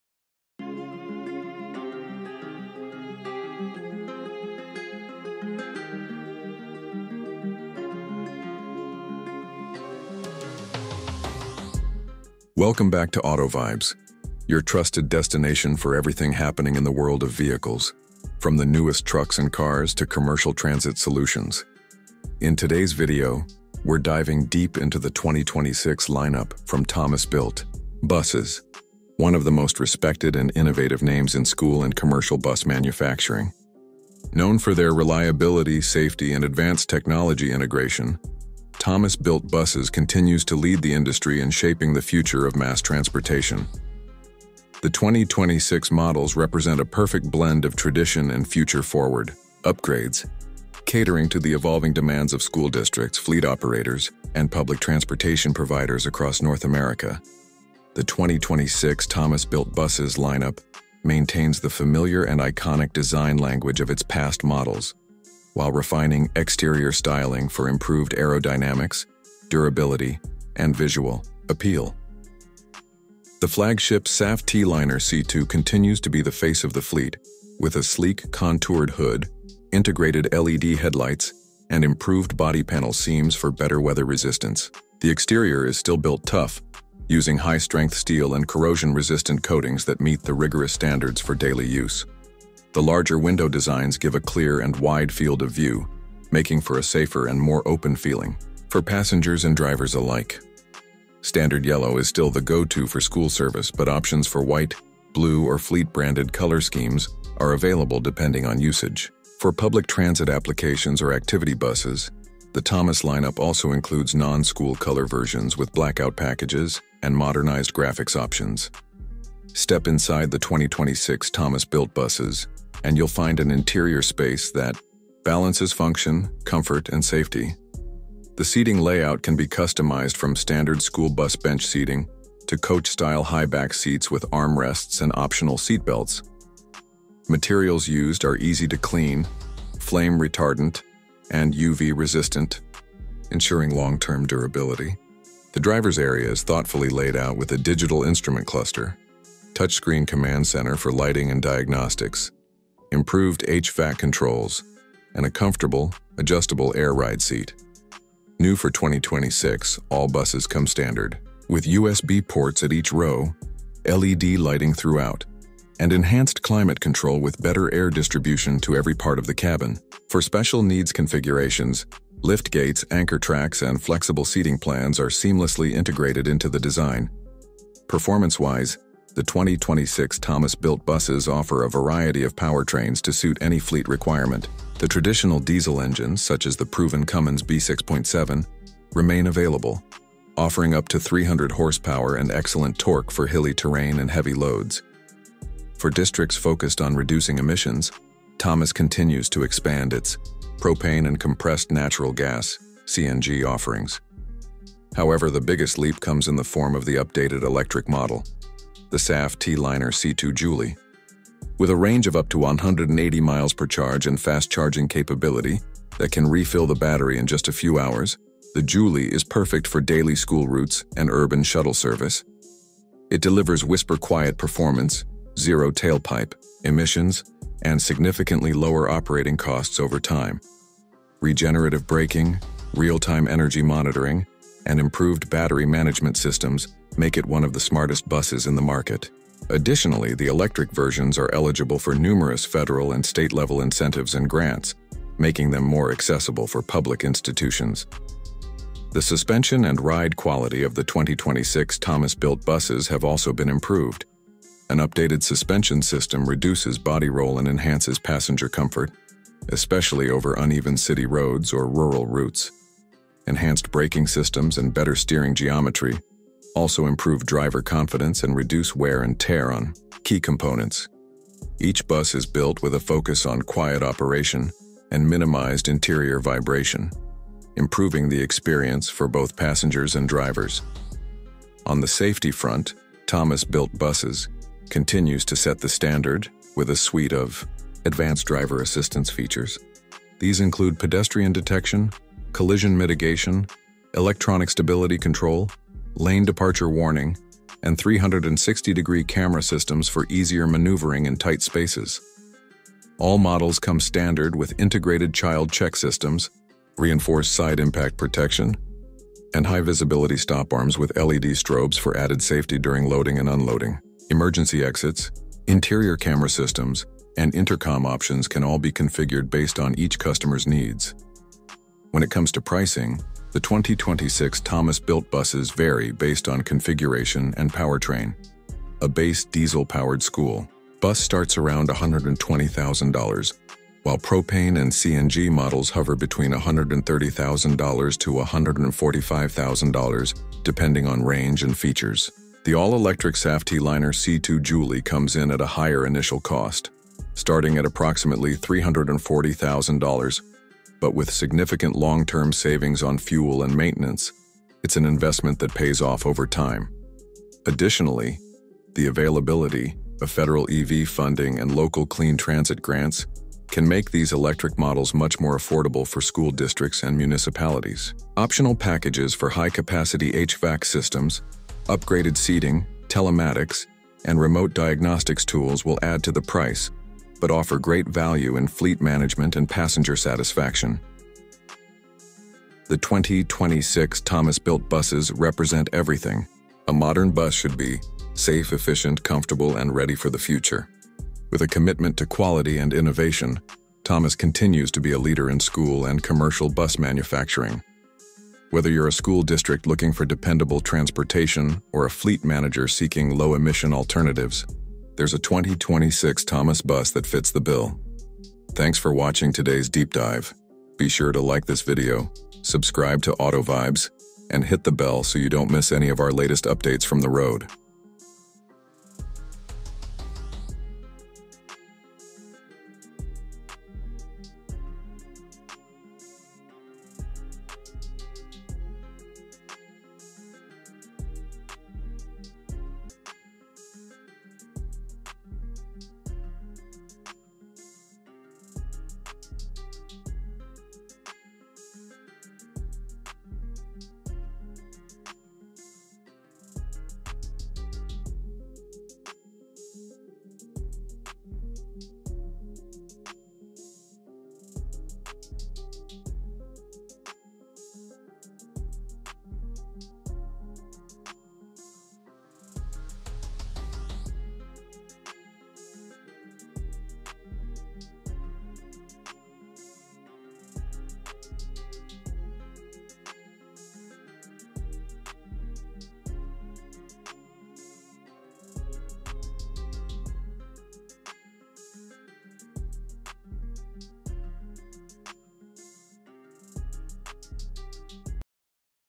2026 Thomas Built Buses – The Future of School Transport!
2026 Thomas Built Buses – The Future of School Transport!

The legendary Thomas Built Buses brand continues to lead the way in school and commercial bus innovation with the all-new 2026 lineup. Trusted for over a century, Thomas Built continues to focus on student safety, driver comfort, and green technology with its updated Saf-T-Liner series, including the popular C2, HDX, and Jouley electric bus.

In today’s video, we explore everything new in the 2026 Thomas Built Buses:
✅ Bold new exterior styling with aerodynamic efficiency
✅ Available in diesel, propane, CNG, and fully electric (Jouley EV)
✅ Seating configurations up to 81 passengers
✅ Industry-leading safety tech: pedestrian detection, automatic braking, 360° cameras
✅ Interior updates with LED lighting, better insulation & HVAC
✅ Digital dash cluster with over-the-air diagnostics
✅ Enhanced accessibility: low-step entry, wheelchair lift options
✅ EV range up to 135 miles per charge (Jouley Electric)
✅ Improved TCO and reduced emissions for schools & districts

Whether for daily student transport, special needs services, or transitioning to a zero-emission fleet, the 2026 Thomas Built Buses are ready to drive the future — safely and sustainably.

💬 Would you go electric with the Thomas Jouley EV? Or stick with clean diesel or CNG?